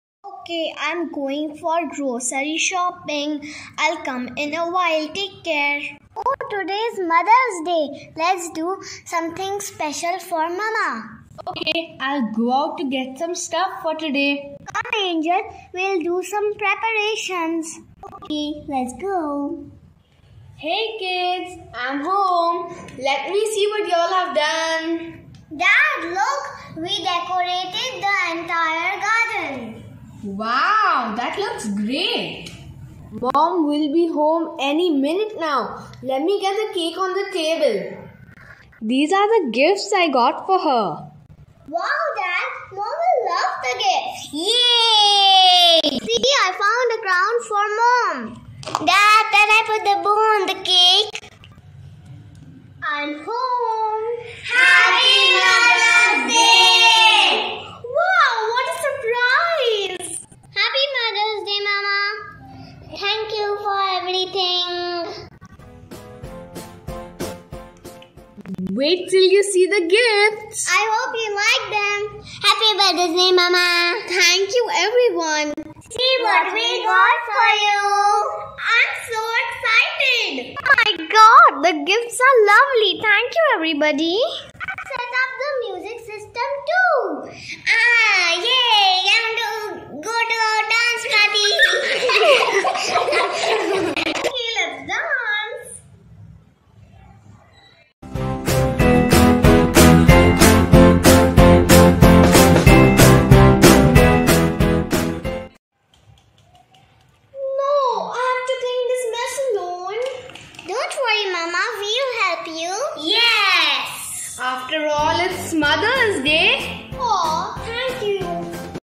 Mother's Day Celebration 🎉❤️
Kids made a set up for mother's day to encourage everyone to make every mother out there feel special.